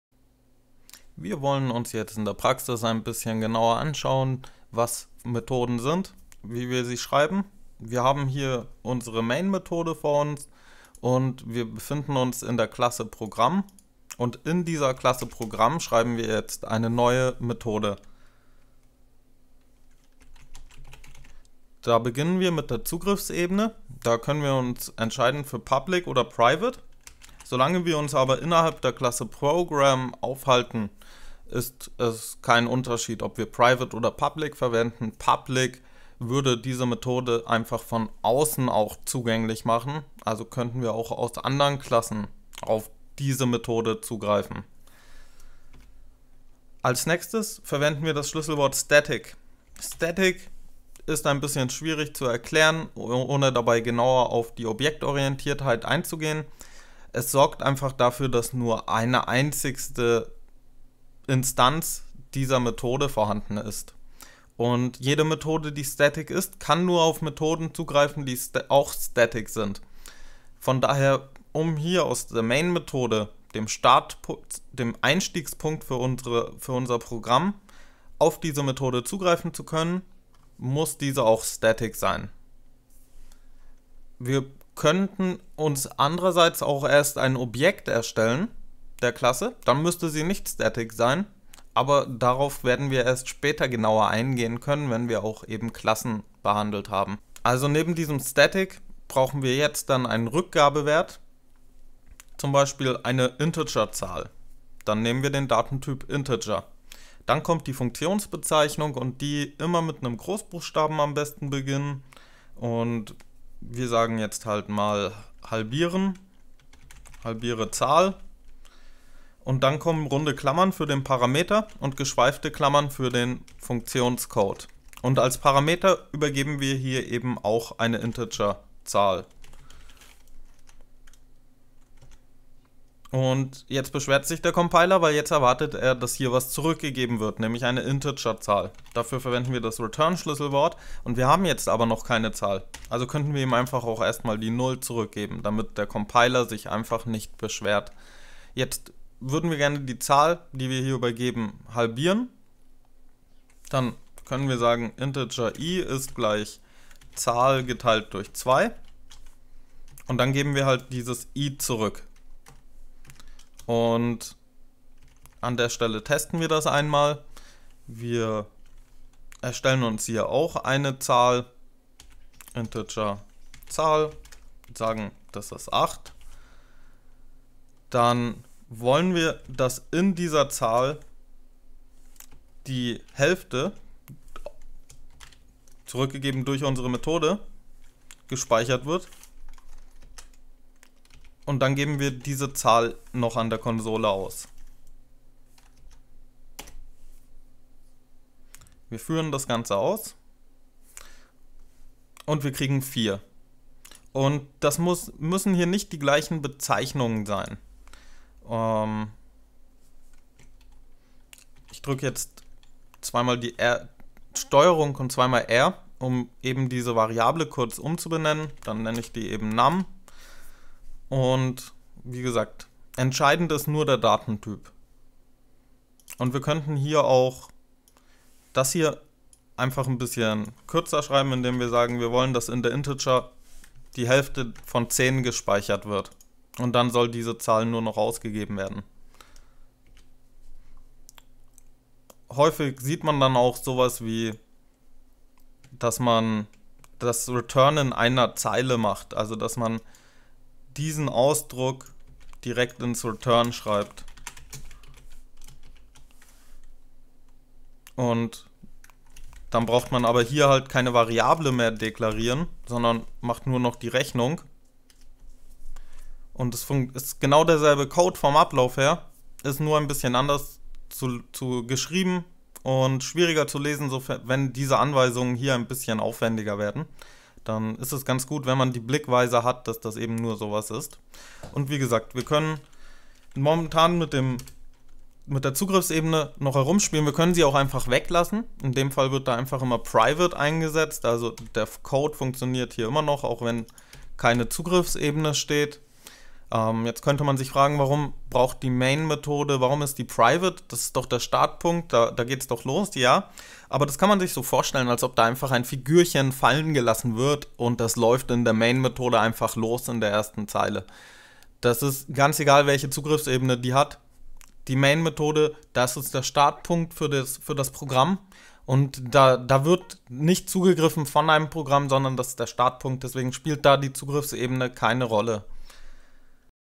C# für Beginner - 13: Funktionen Demo
Eine kurze Demonstration einer Methoden-Deklaration. Wir erstellen eine einfache Methode die eine übergebene Zahl halbiert und lernen das return Schlüsselwort kennen.